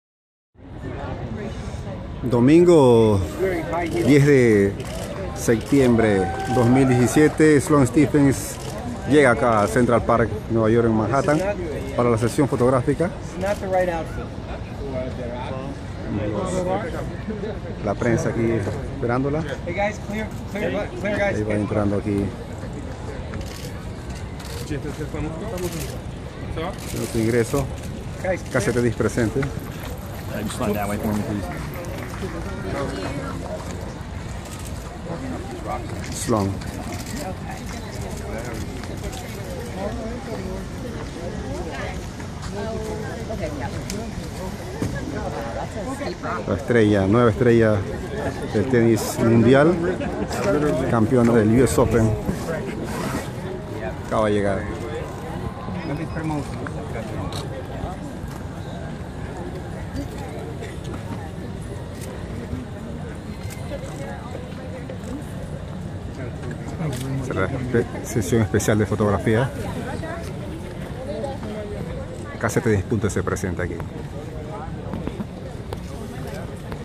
US Open Sloane Stephens sesión oficial de fotos
La nueva estrella del tenis mundial, Sloane Stephens, llega al Central Park para la sesión oficial de fotos, tal cual una estrella de Hollywood. Radiante la campeona del US Open 2017. Cobertura desde Nueva York gracias a SURTAX, Guantanamera Restaurant New York, Clínica de Medicina deportiva Dr Tyrone Flores y (NYC septiembre 10 de 2017).